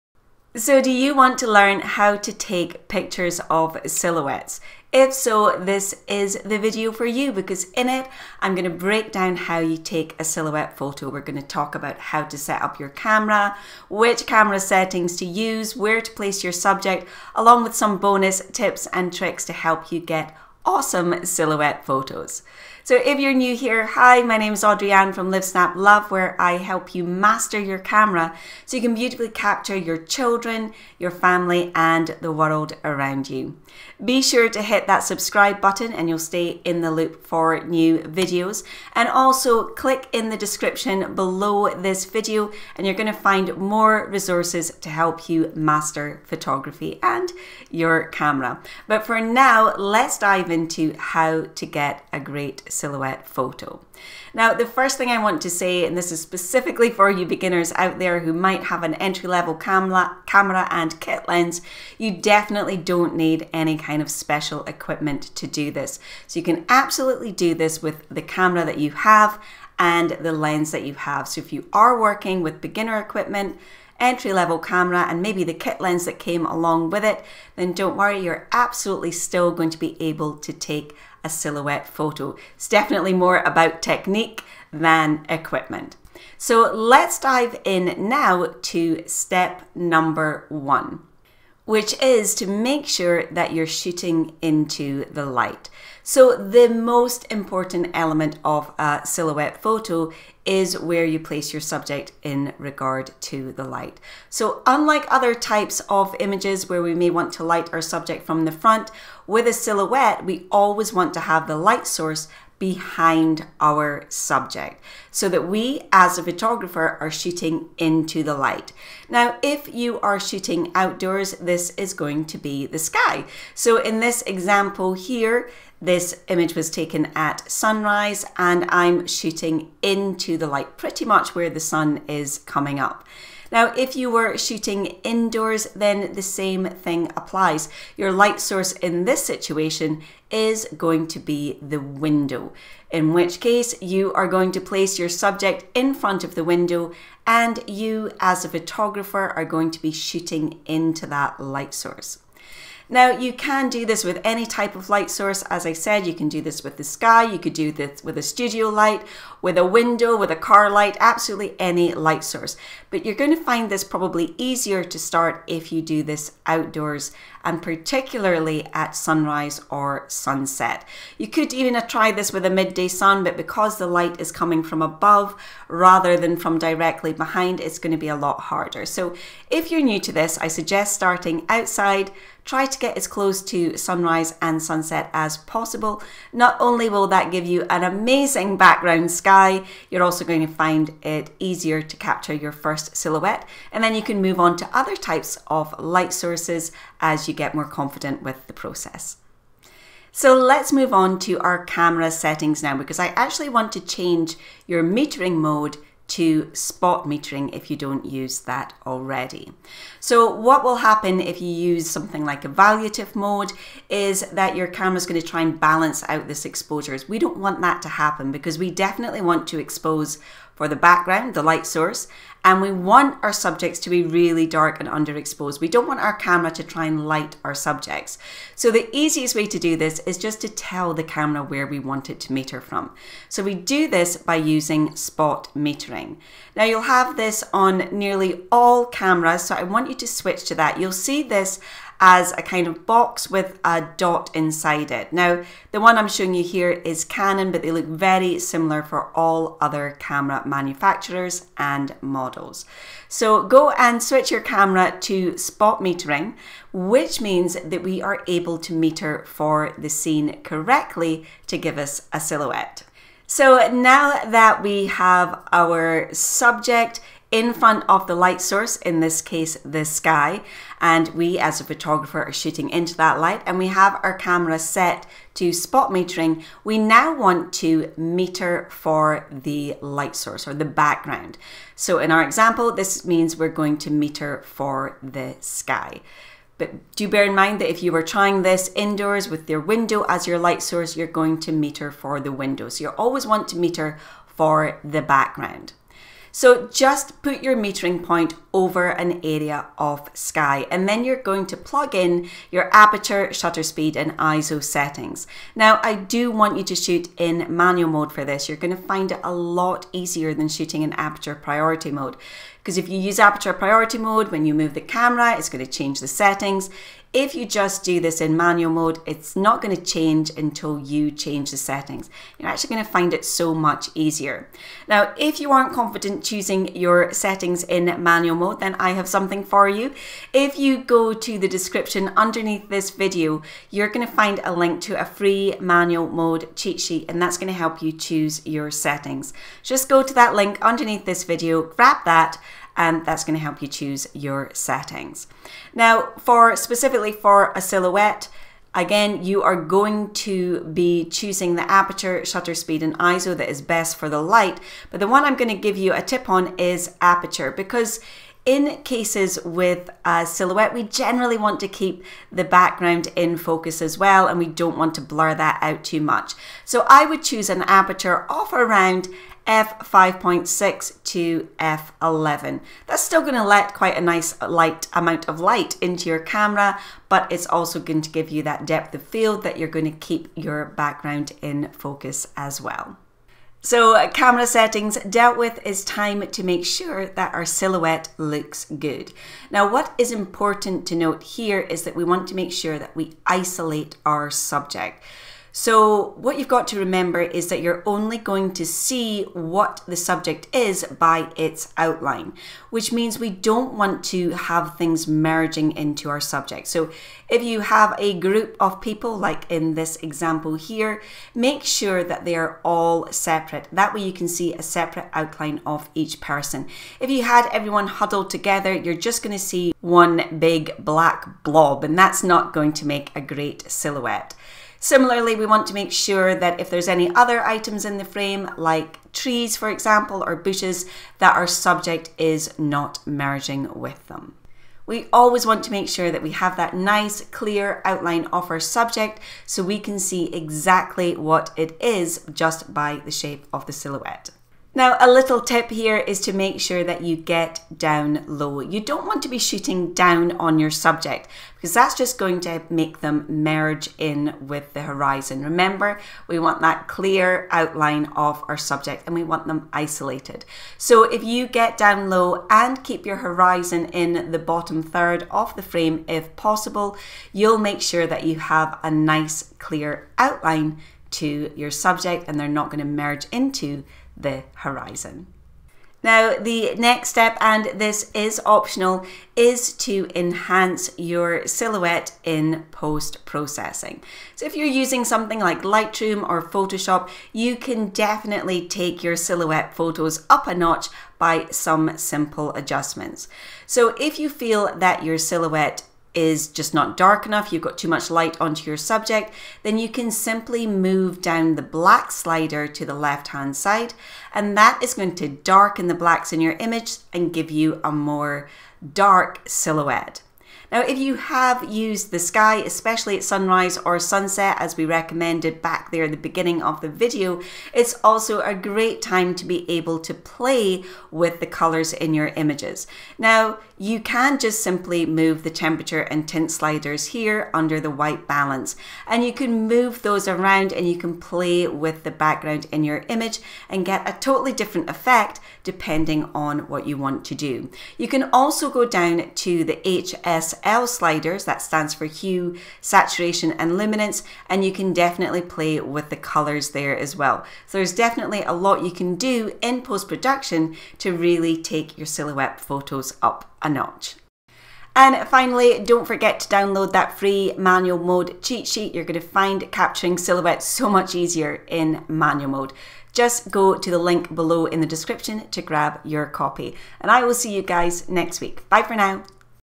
Silhouette Photography : How to Take Pictures of Silhouettes
Silhouette Photography // Learn how to take pictures of silhouettes in this easy to understand, step by step photography tutorial!  *** FREE GUIDE! 👉Manual Mode Cheat Sheet

-- LINKS & RELATED CONTENT --

-- SILHOUETTE PHOTOGRAPHY --

In this photography tutorial, I’m going to show you how to photograph silhouettes in just 5 easy steps!  We’ll cover what settings to use, where to place your subject, along with tips and tricks that will help you get an awesome silhouette photo!